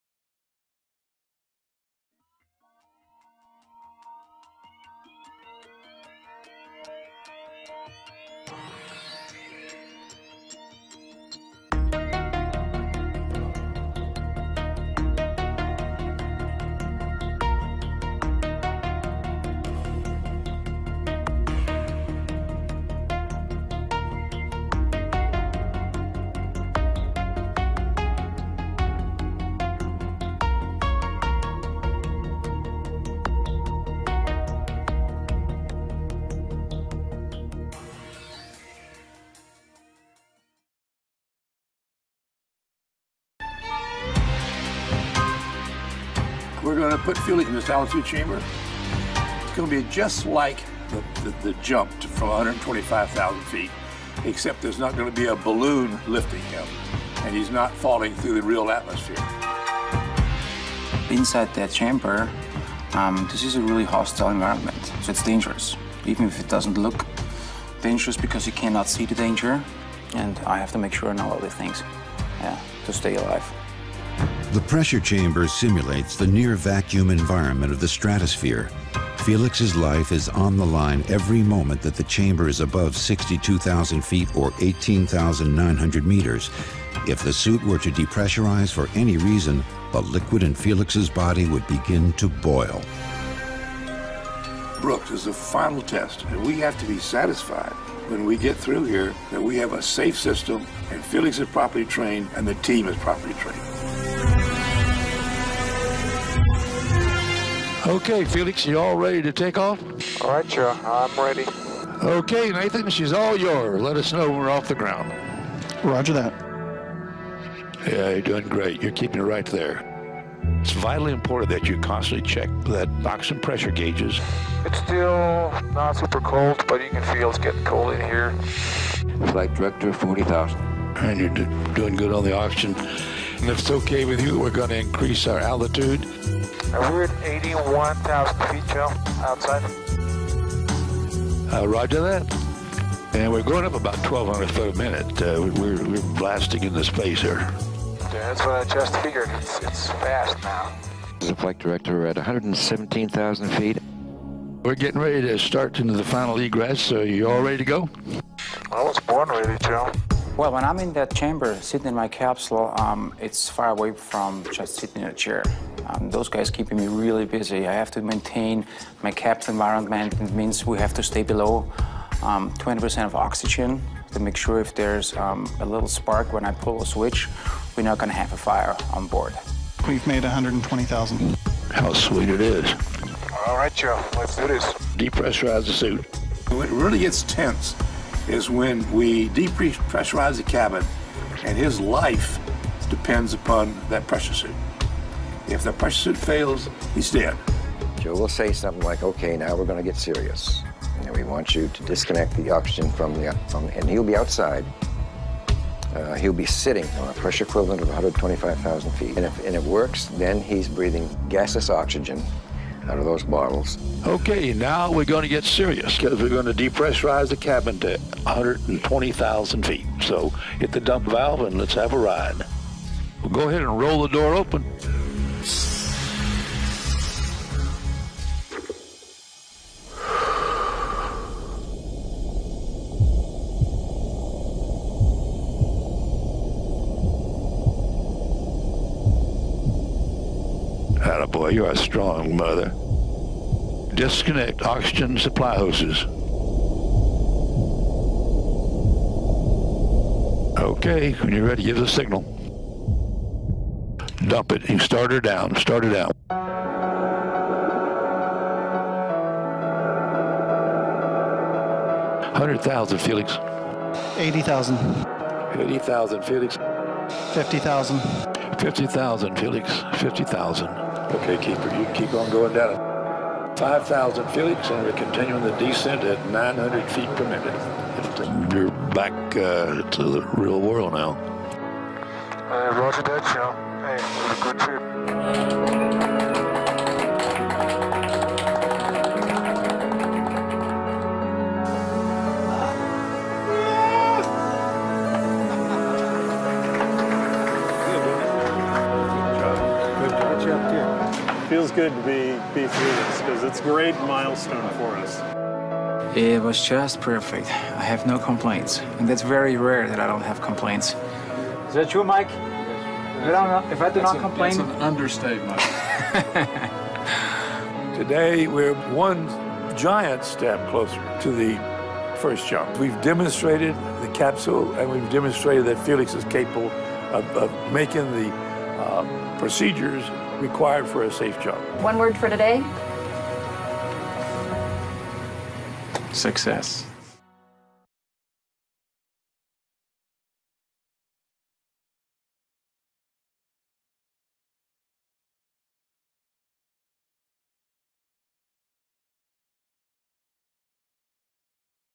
Team Stellar News - Red Bull Stratos Project - Altitude Chamber Testing
Team Stellar's Space News. This episode showcases the Red Bull Stratos Project. Two of Team Stellar's members designed and built the electrical systems onboard the Red Bull Stratos Capsule. The mission, to bring skydiver Felix Baumgartner to 120,000 feet to break several world records including the highest and fastest parachute jump, breaking the speed of sound.